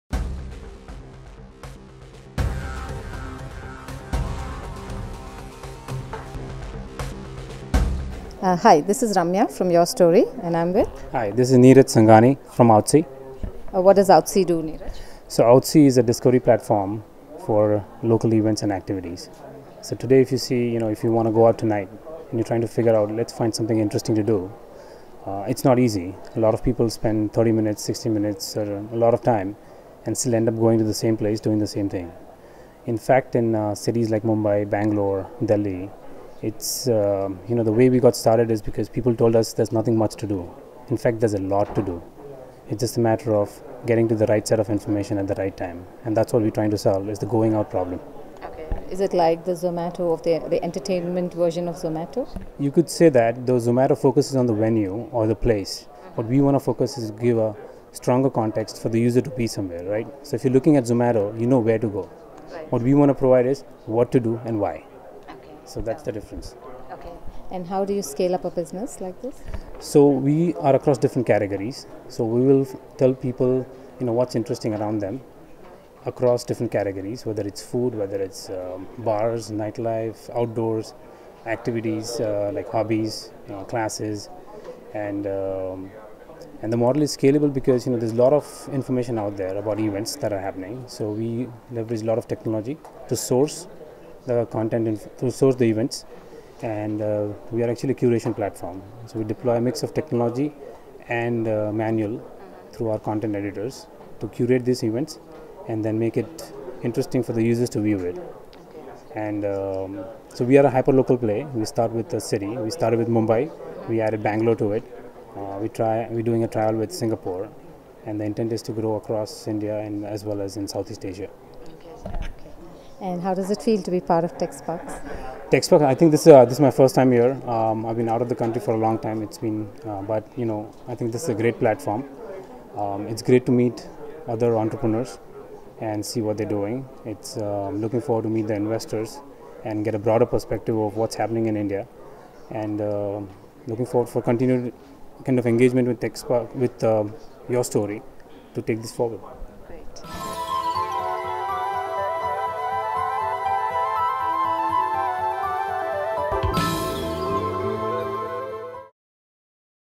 Outsy, Tech30, TechSparks 2014, YourStory.com
Outsy is a discovery platform for local events. It is trying to solve for boredom by recommending experiences. Early in the evolution still, the Outsy app has more than 5,000 downloads on Android. They are focusing on Android, iOS and the mobile web.